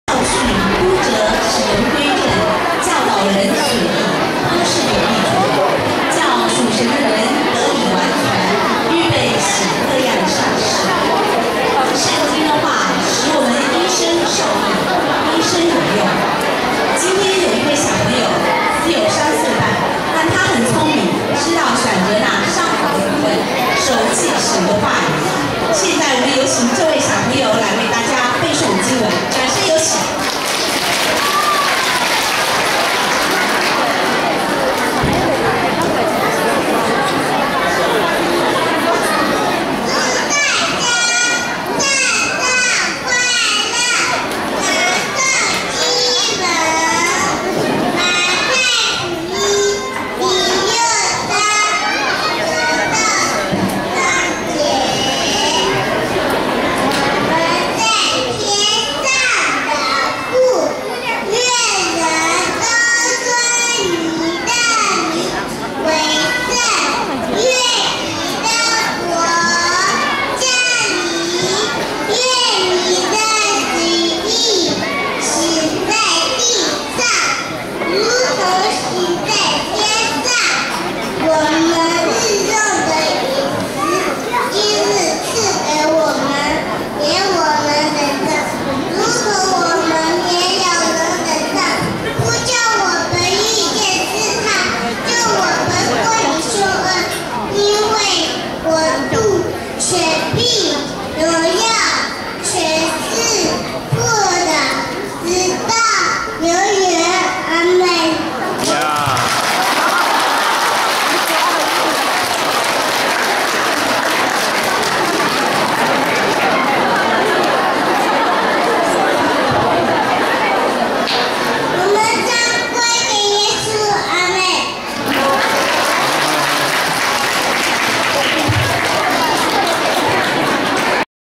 2010年巴黎温州教会12月25日圣诞晚会—04三岁小孩背诵经文
这是一位三岁半的孩子，背诵：马太福音书 6章9-13节 的祷告文